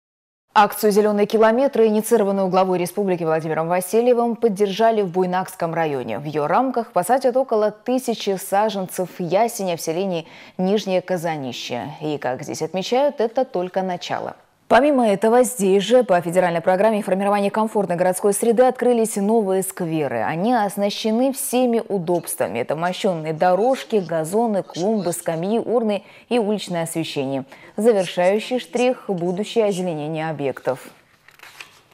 Акцию «Зеленые километры» поддержали в Буйнакском районе
Акцию «Зеленые километры», инициированную главой республики Владимиром Васильевым, поддержали в Буйнакском районе. В ее рамках посадят около 1000 саженце ясеня в селении Нижнее Казанище. И, как здесь отмечают, это только начало.